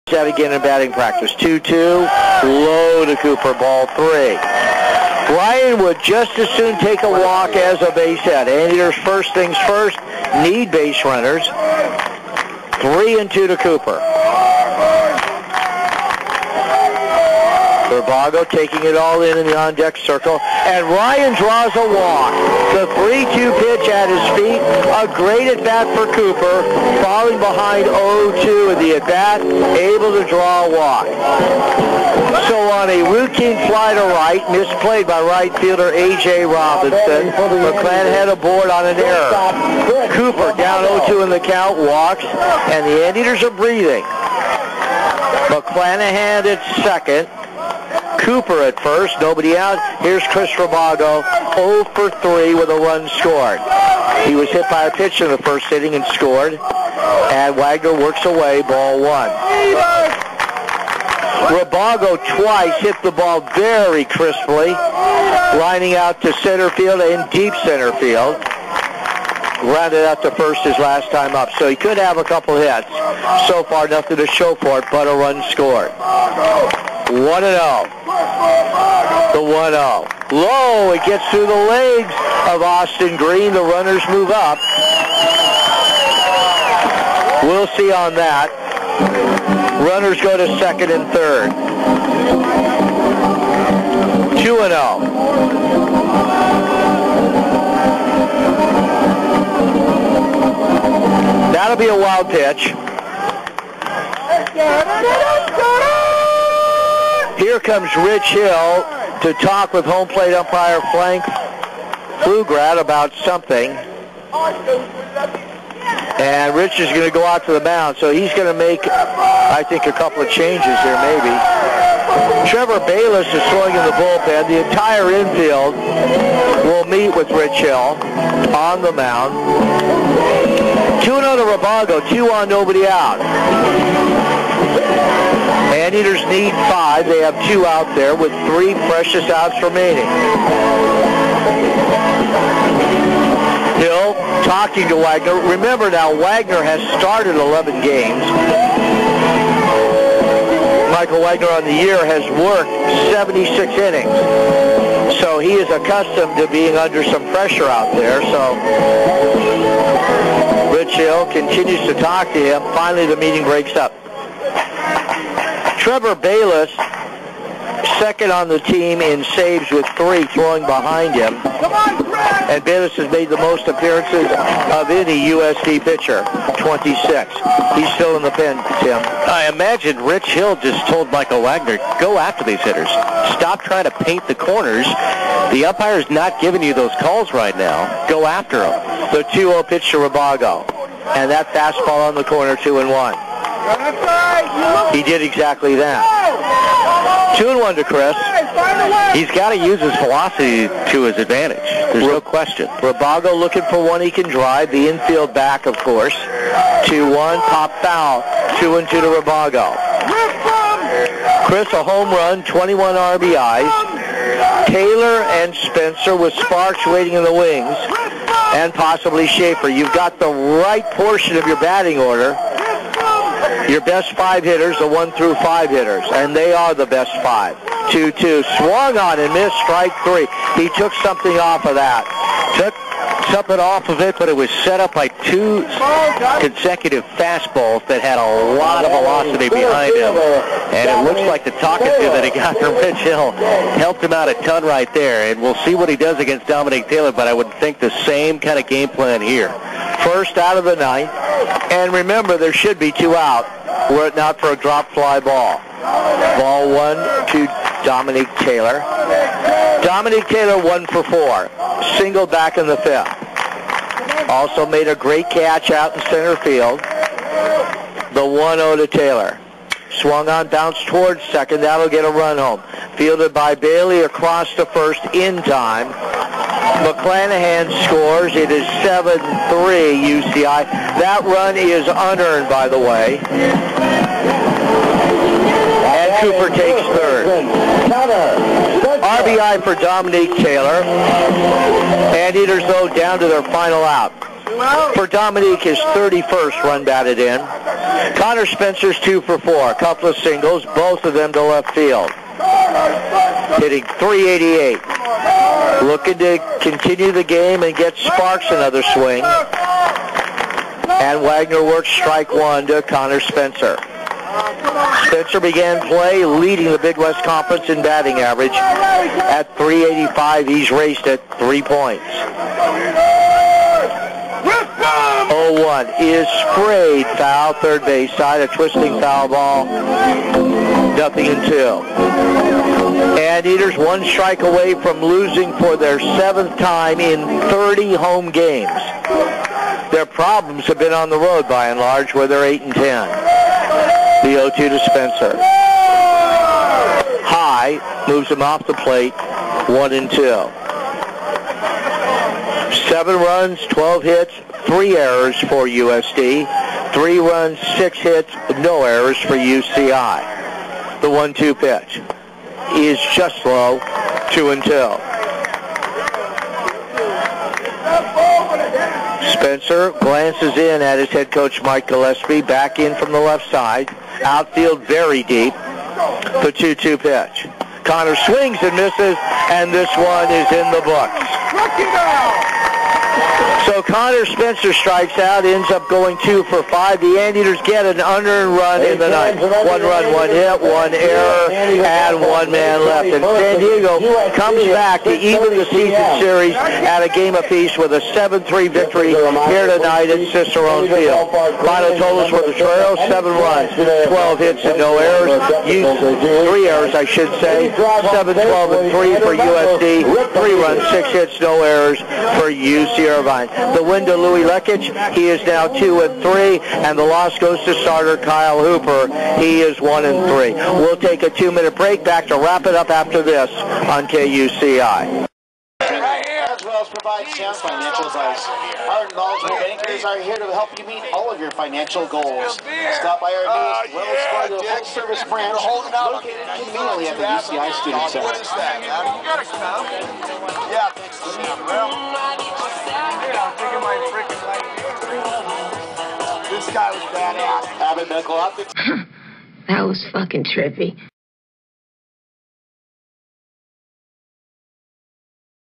UCI Baseball 5.15.13
bottom of the the 9th radio replay with "The Mighty Thor" leading the cheers